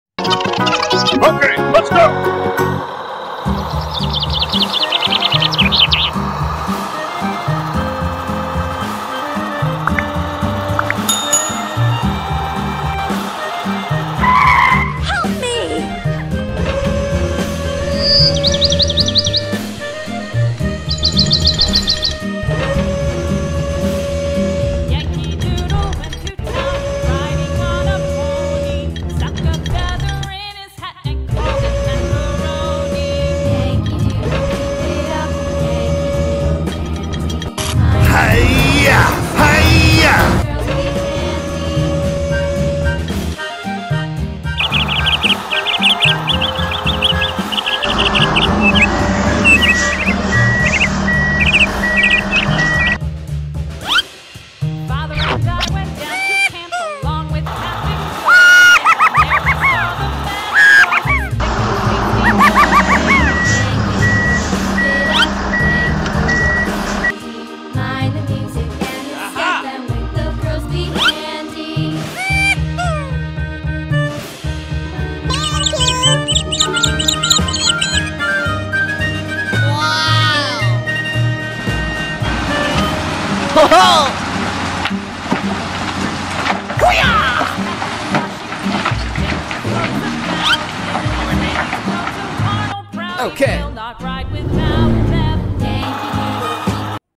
evakuasi mobil truk yg tertimpa reruntuhan||mobil mainan
melakukan penyelamatan mobil truk yg tertimpa reruntuhan.mobil truk mainan traktor mainan ekskavator mobil tangki mobil bus,miniatur anak anak.